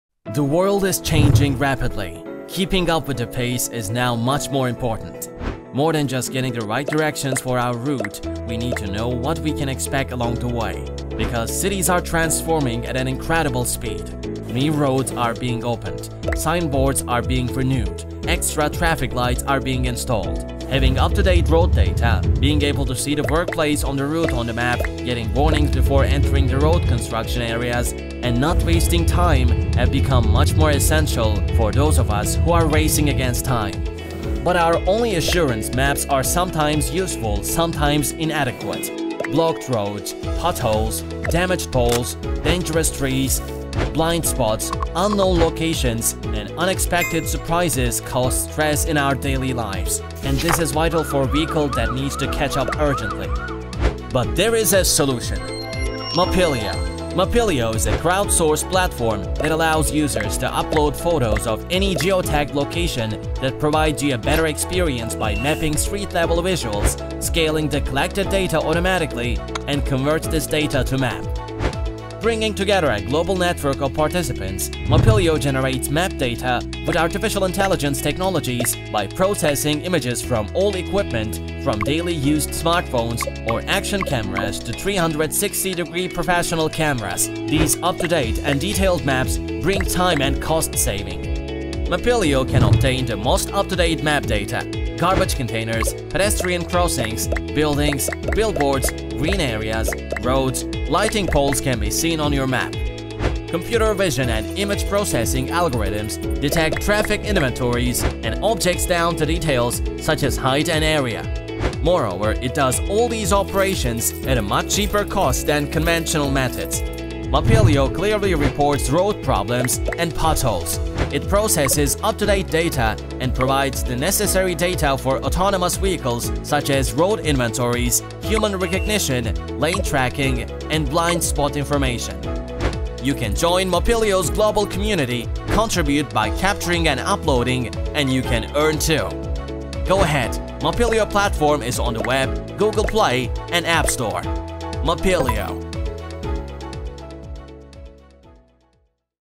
Mapilio Platform
Mapilio is the platform that makes street-level images and map data available to scale and automate mapping.